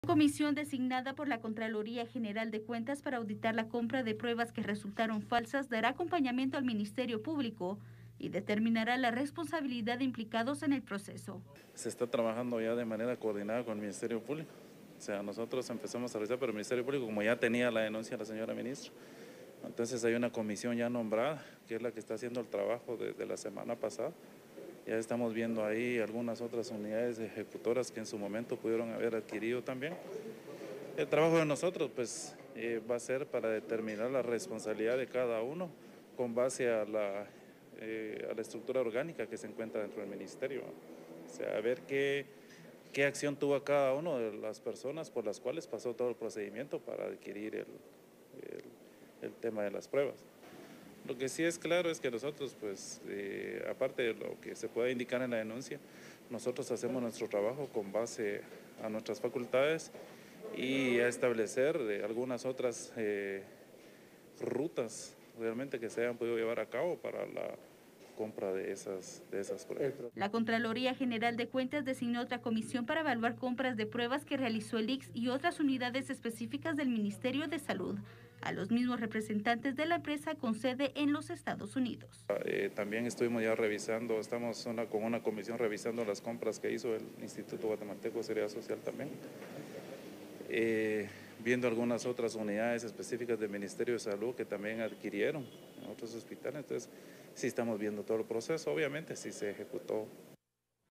Contraloría auditará adquisición de pruebas de Covid 19 que resultaron falsas
la CGC dará acompañamiento al MP para determinar los responsables de la compra anómala

La Contraloría General de Cuentas estará brindando colaboración con el Ministerio Público para determinar quiénes son los responsables de la compra anómala de pruebas Covid 19.